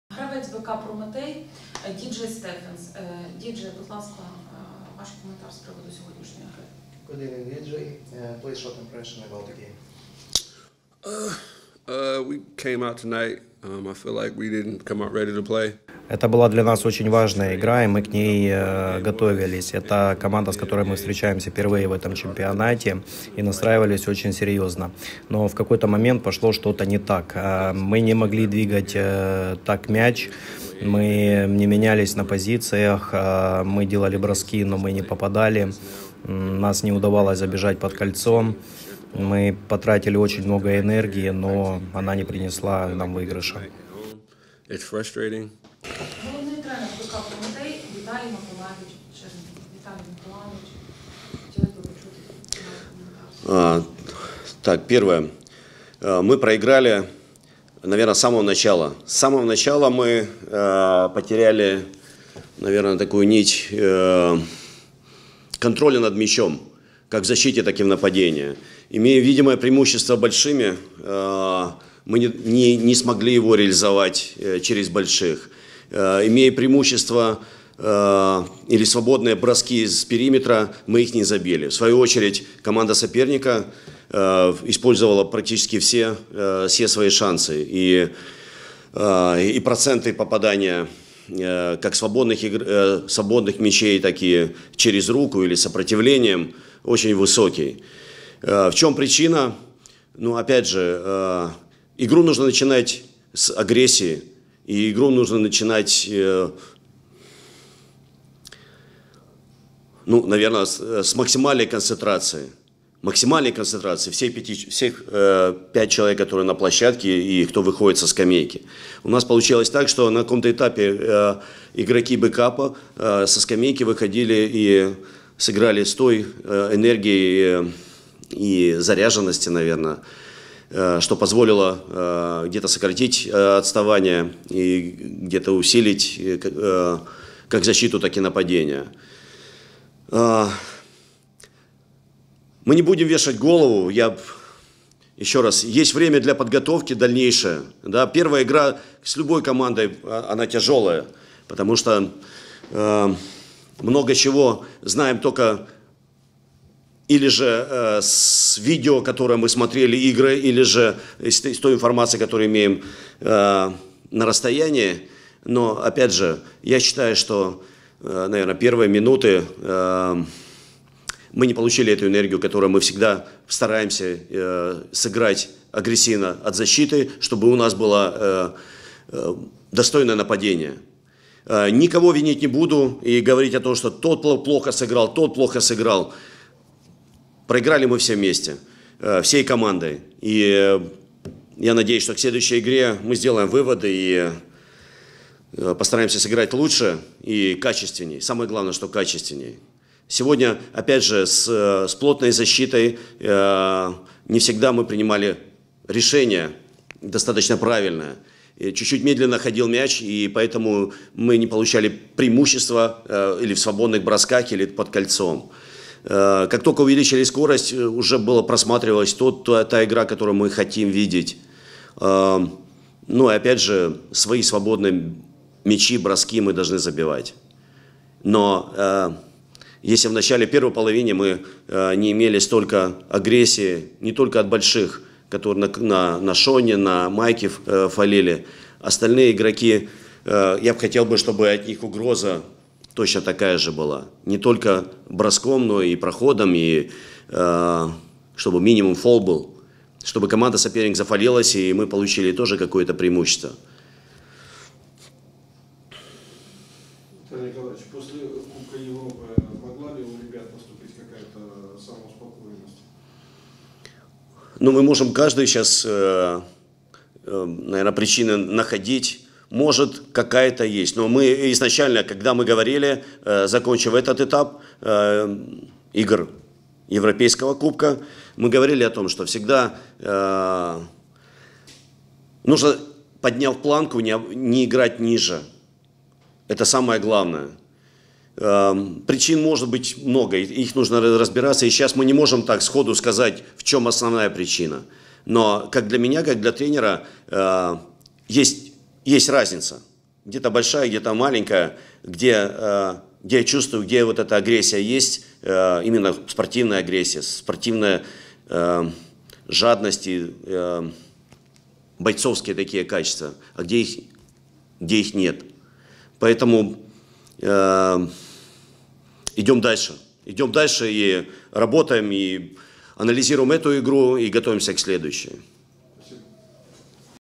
БК "Прометей" - БК "Будівельник": післяматчеві коментарі "Прометея"
БК "Прометей" - БК "Будівельник": післяматчеві коментарі "Прометея". Суперліга Паріматч. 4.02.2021